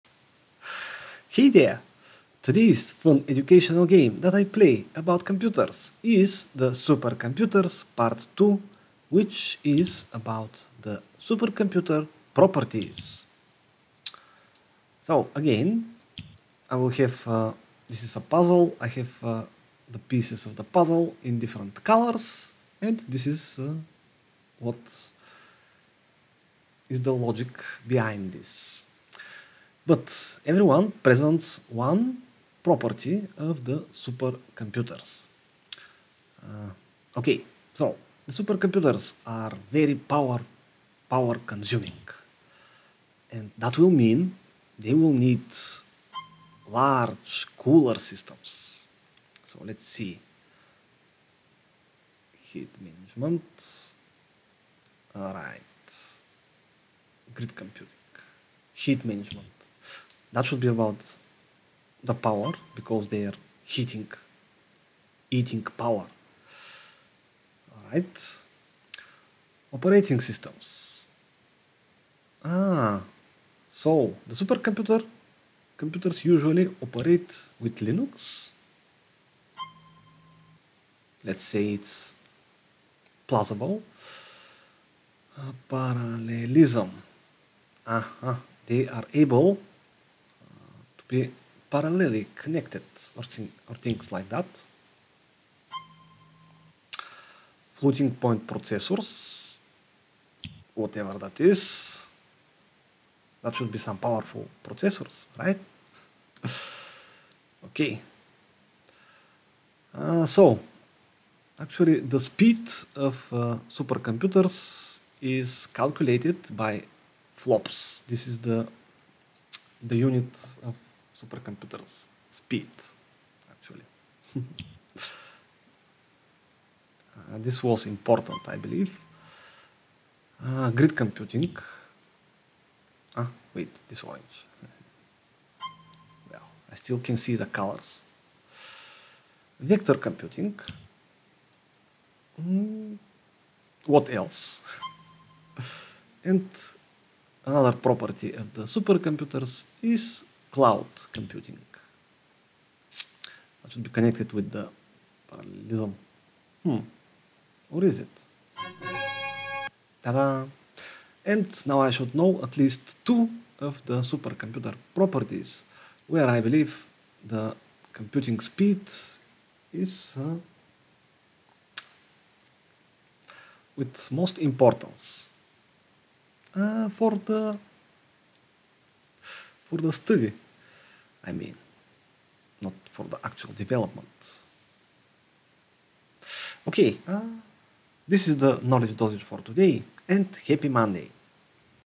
Supercomputer Properties - Interactive IT Lesson 💻🎥🎓
Fun Information Technology class with master Ogiwon about the Supercomputer advantages and disadvantages. Suitable for online lessons, interactive classes and exciting homework.